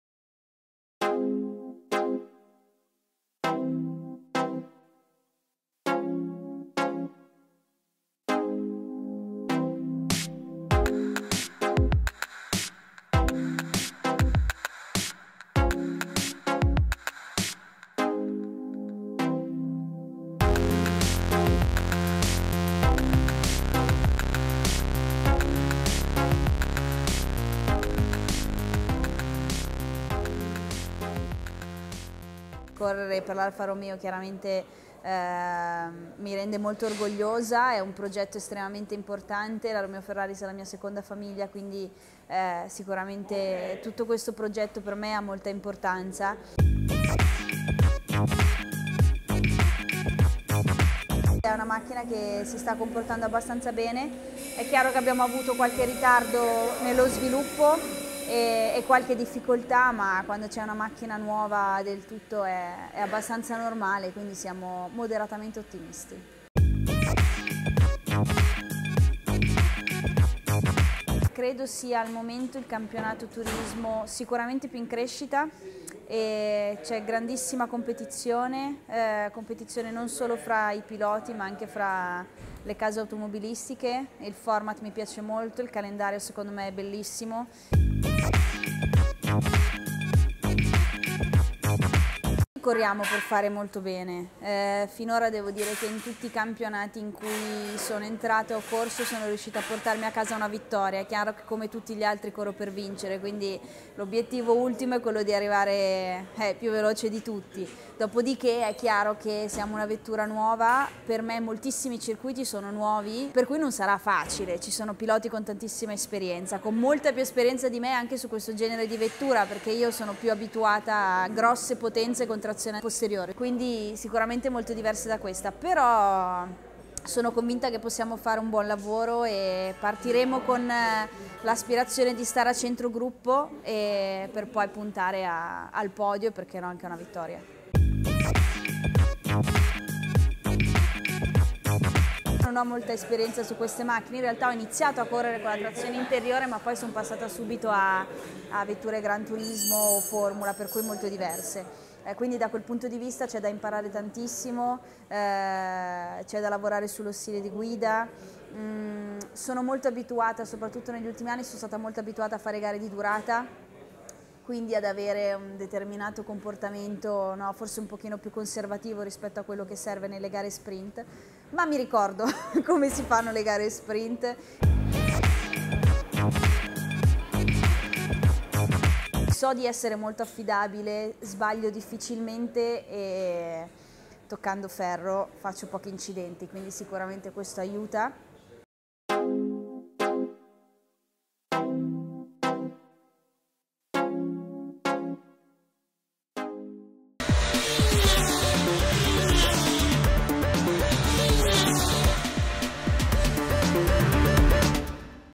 2016 Valencia, TCR Alfa Romeo Giulietta on track
Michela Cerruti speaks about her expectations for the 2016 TCR campaign